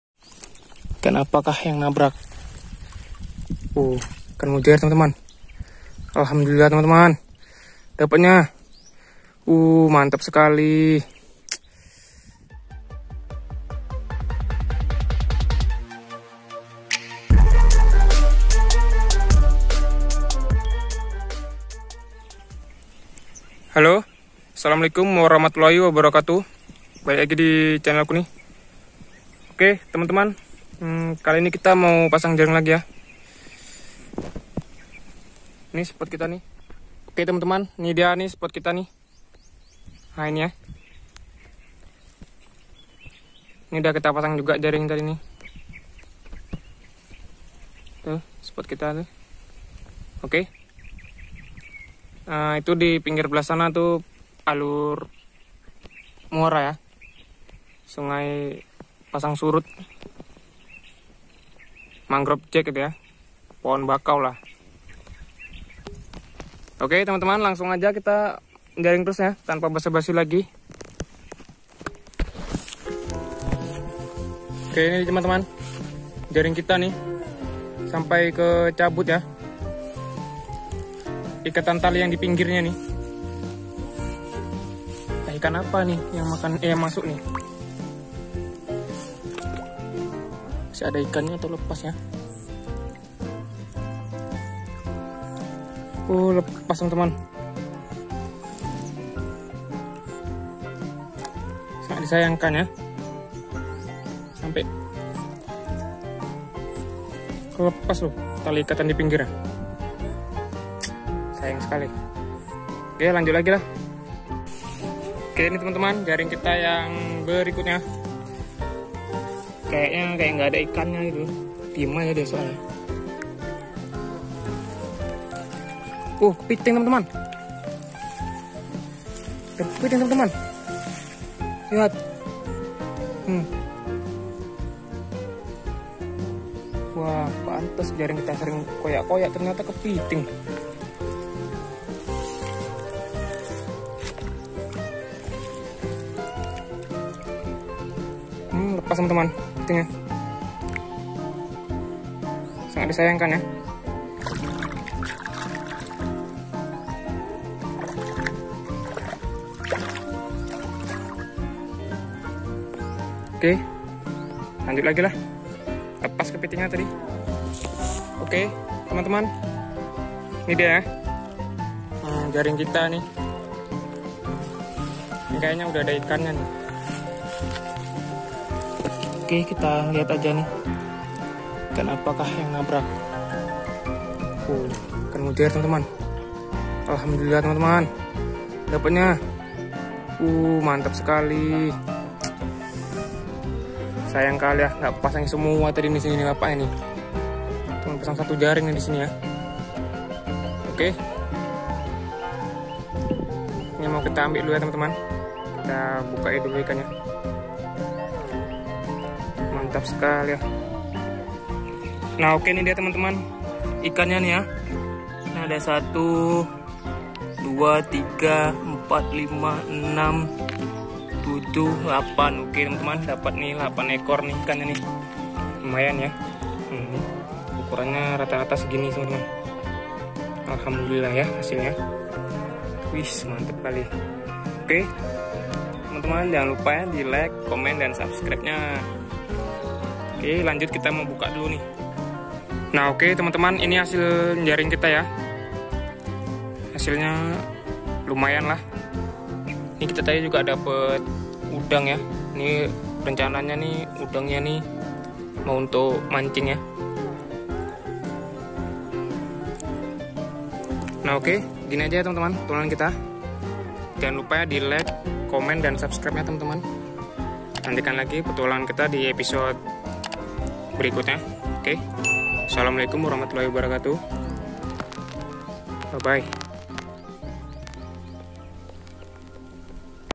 JARING IKAN NILA BIKIN KAGET!! ADA KEPITINGNYA || jaring 2 inch
jaring ikan nila di sungai hampir dapat kepiting
lokasi kebun sawit r4